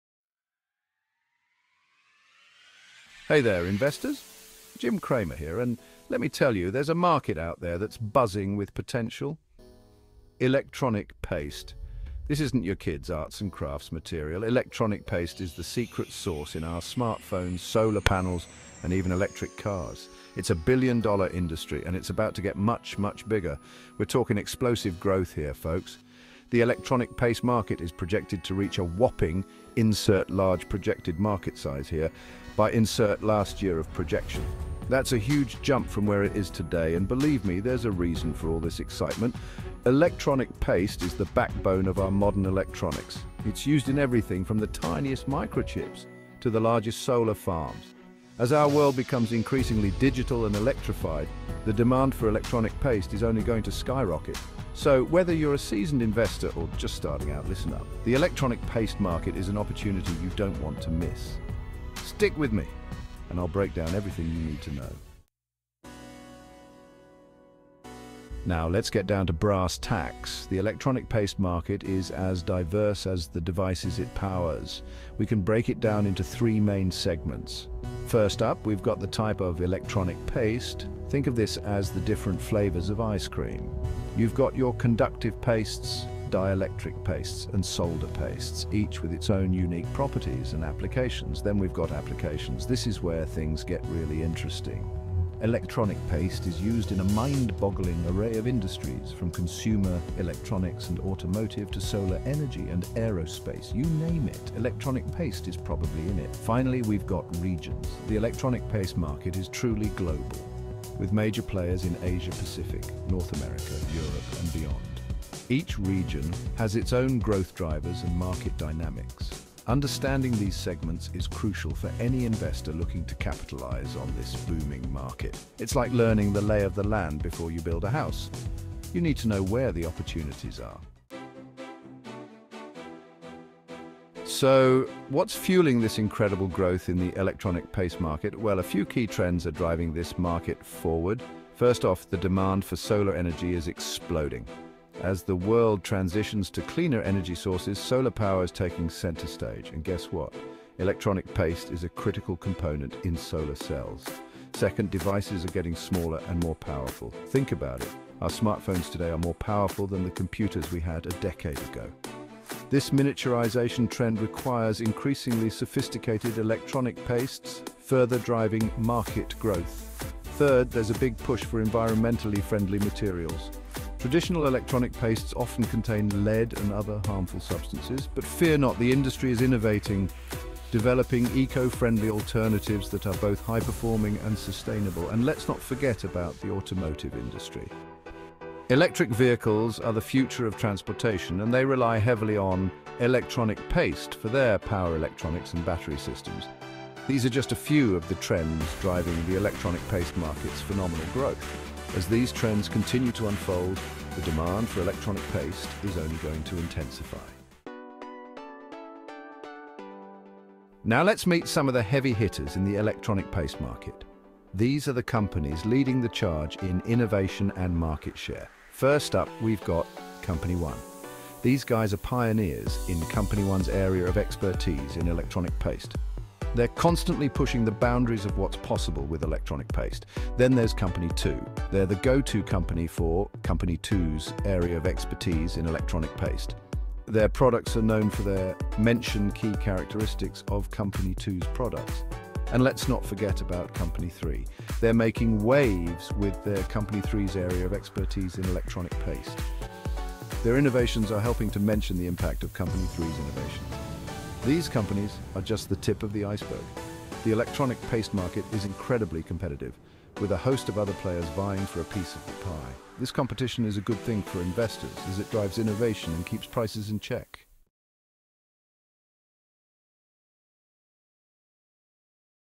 Electronic Paste Market Research Report
The global Electronic Paste market is projected to grow from US$ 5836.7 million in 2024 to US$ 9967.9 million by 2030, at a Compound Annual Growth Rate (CAGR) of 9.3% during the forecast period.